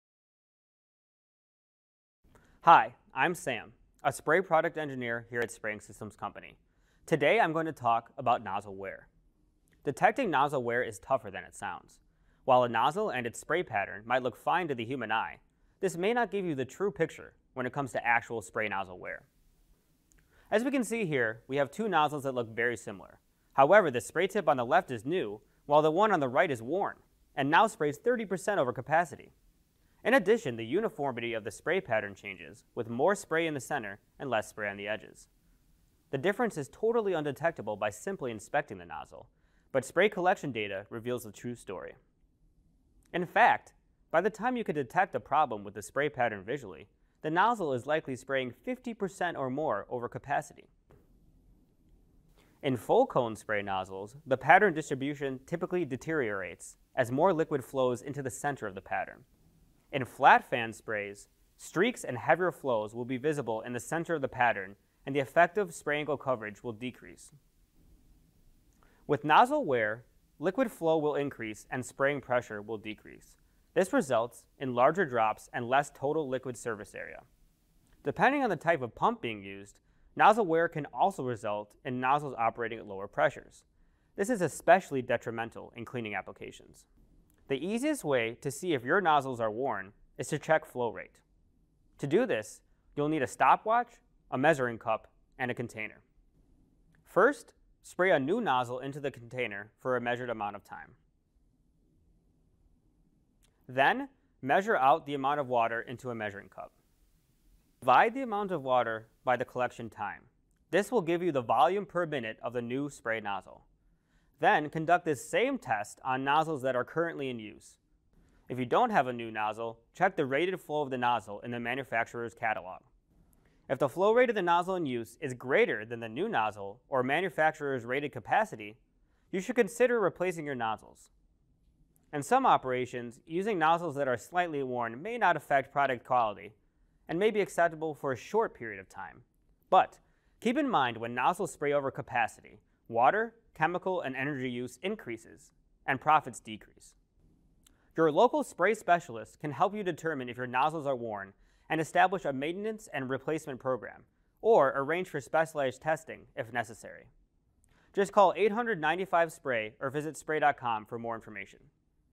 How to Detect Nozzle Wear
Detecting nozzle wear is tougher than it sounds. While a nozzle might look fine to the human eye, this may not give you the true picture when it comes to actual spray nozzle wear. There are several indicators of nozzle wear.
Worn, clogged or damaged spray nozzles will not perform per specification and can result in uneven coating, cooling, cleaning, humidifying, and drying. This usually leads to Quality control issues and increased scrap. The flow rate of a spray nozzle will increase as the surfaces of the orifice and/or the internal core begin to wear. Nozzles will spray over capacity and the result is wasted water, chemicals and energy.